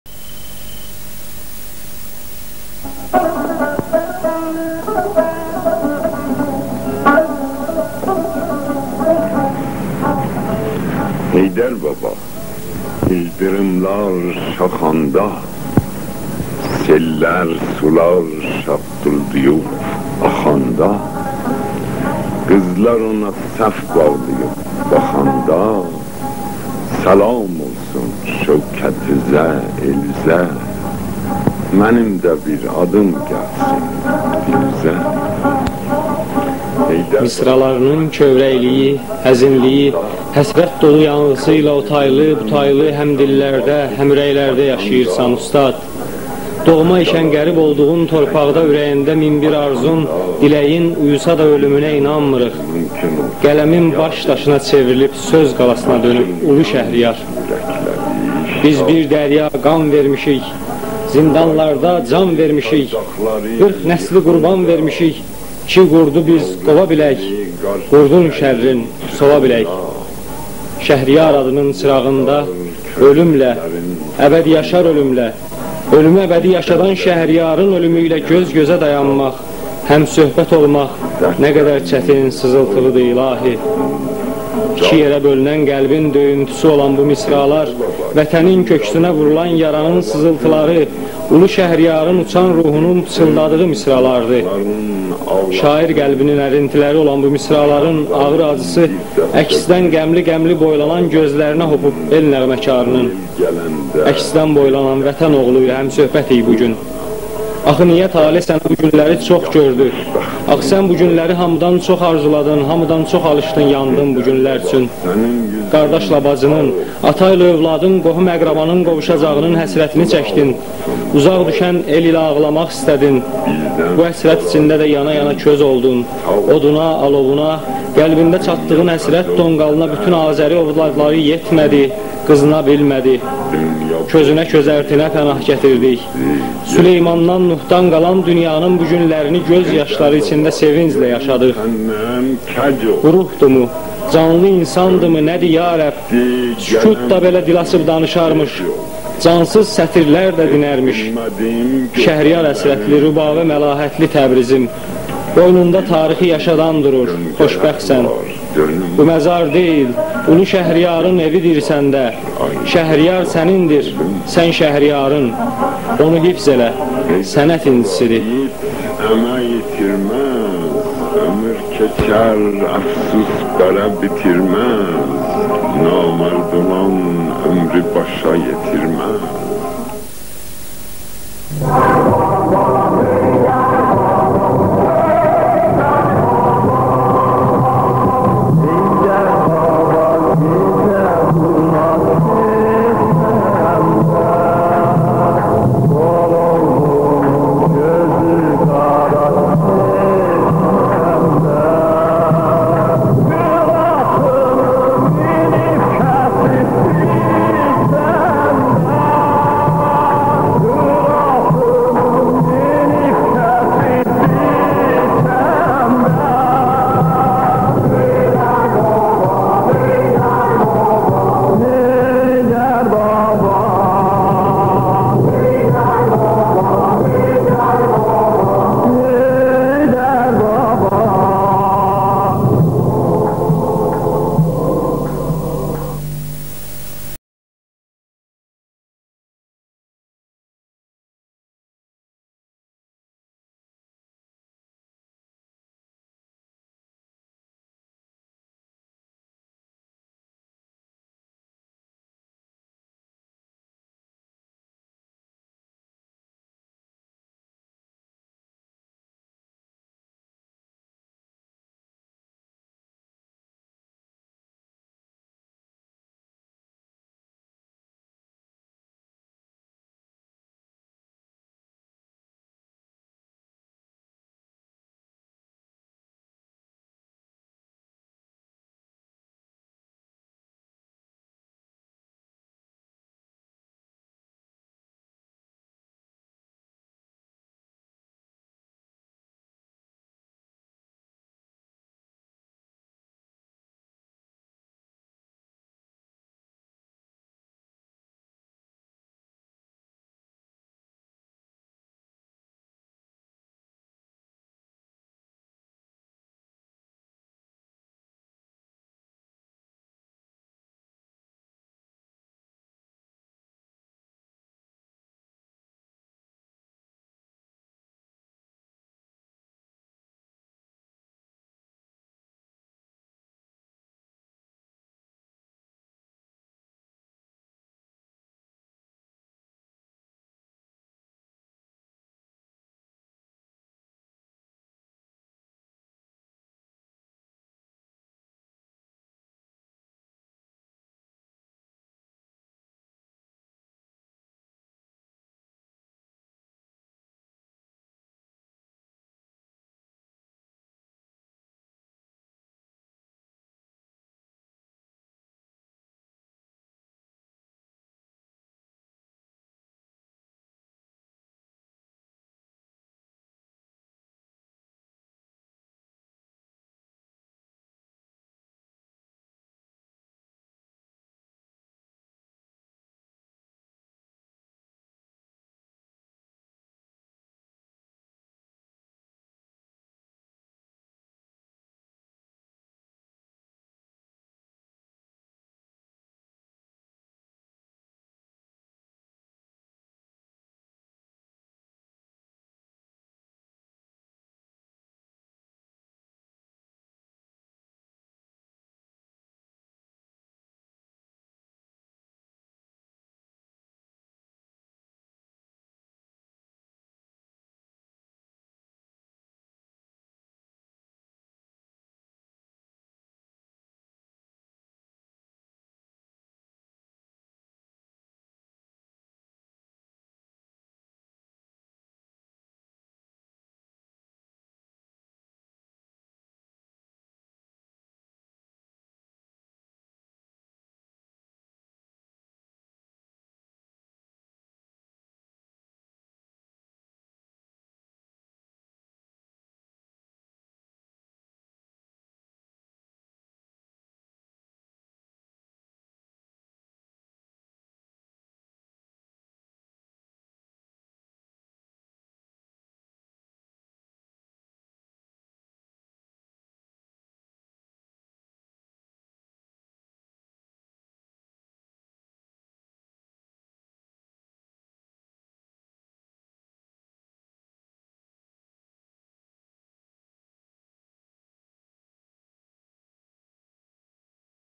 Rahib Qəribin arxivindən: 1992-ci ildə AzTV-də Məhəmmədhüseyn Şəhriyar haqda efirə verilən süjetim.
Biz bir dərya qan vermişik,
Zindanlarda can vermişik,
Qırx nəsli qurban vermişik,
Ki qurdu biz qova bilək,
Qurdun şərrin sova bilək